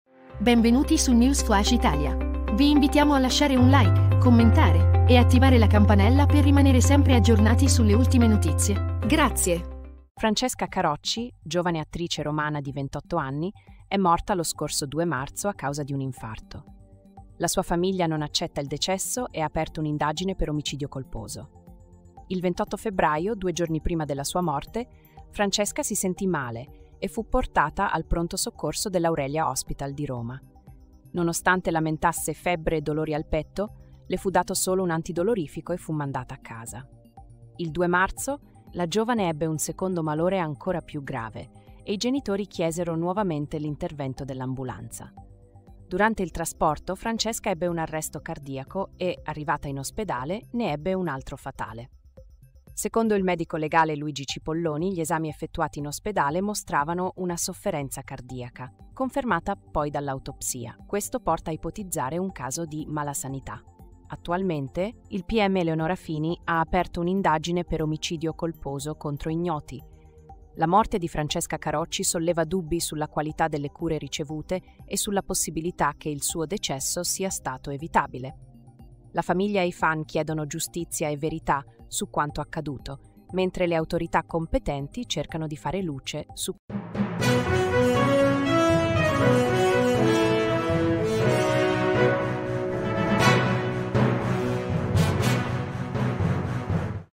🚨FRANCESCA CAROCCI: MORTA A 28 ANNI PER MALASANITÀ OSPEDALIERA
Francesca Carocci, giovane attrice romana di 28 anni, è morta lo scorso 2 marzo a causa di un infarto. La sua famiglia non accetta il decesso e ha aperto un'indagine per omicidio colposo.

Il 28 febbraio, due giorni prima della sua morte, Francesca si sentì male e fu portata al pronto soccorso dell'Aurelia Hospital di Roma. Nonostante lamentasse febbre e dolori al petto, le fu dato solo un antidolorifico e fu mandata a casa. Il 2 marzo, la giovane ebbe un secondo malore ancora più grave e i genitori chiesero nuovamente l'intervento dell'ambulanza. Durante il trasporto, Francesca ebbe un arresto cardiaco e, arrivata in ospedale, ne ebbe un altro fatale.

Secondo il medico legale Luigi Cipolloni, gli esami effettuati in ospedale mostravano una sofferenza cardiaca, confermata poi dall'autopsia. Questo porta a ipotizzare un caso di malasanità. Attualmente, il pm Eleonora Fini ha aperto un'indagine per omicidio colposo contro ignoti.

La morte di Francesca Carocci solleva dubbi sulla qualità delle cure ricevute e sulla possibilità che il suo decesso sia stato evitabile. La famiglia e i fan chiedono giustizia e verità su quanto accaduto, mentre le autorità competenti cercano di fare luce su questa tragica vicenda.